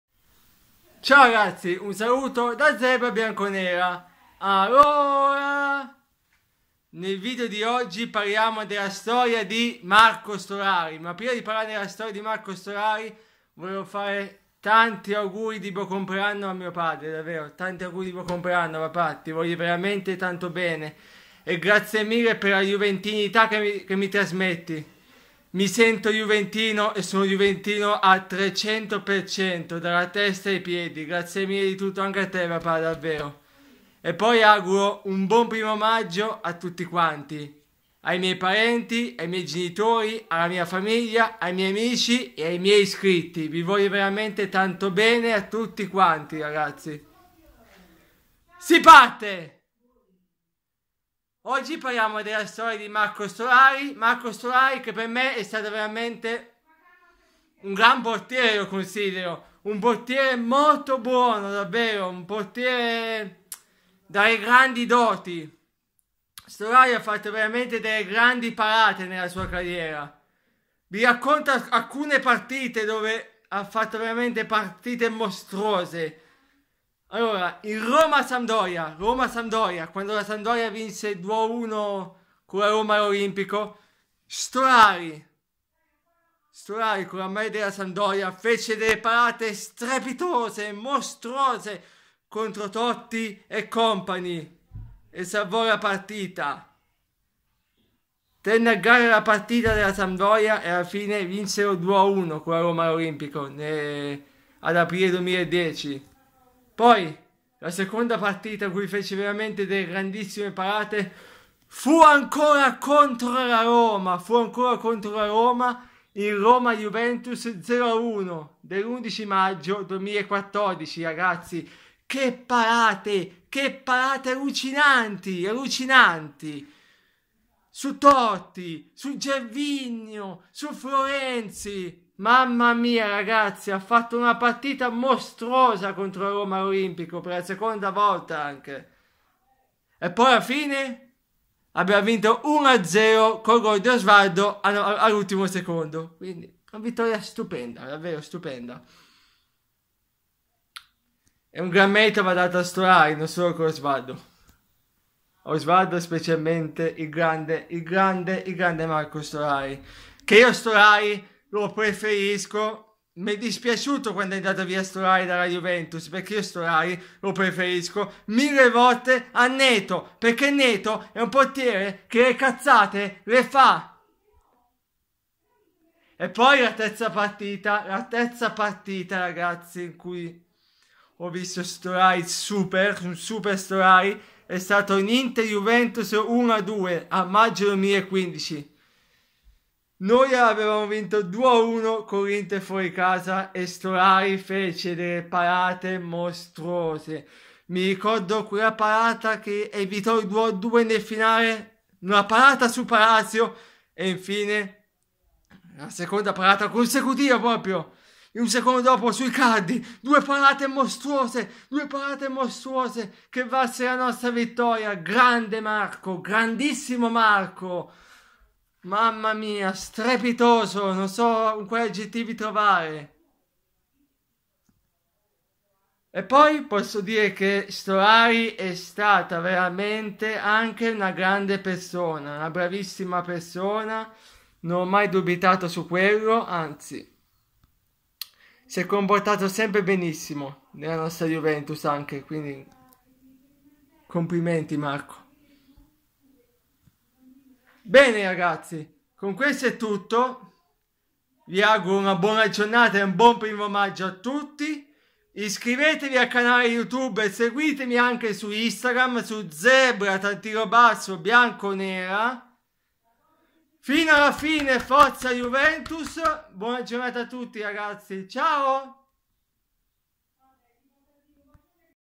LA STORIA DI MARCO STORARI
In questo video parliamo di Marco Storari con alcune delle sue parate mostrouse in alcune partite in questi anni, specialmente con la maglia bianconera